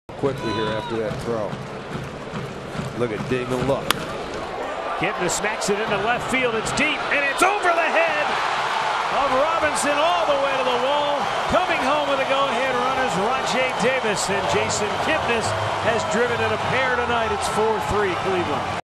LAA@CLE: Kipnis doubles to left, scores Davis in 5th
Jason Kipnis lines an opposite-field double over the left fielder's head, scoring Rajai Davis to give the Indians a 4-3 lead in the 5th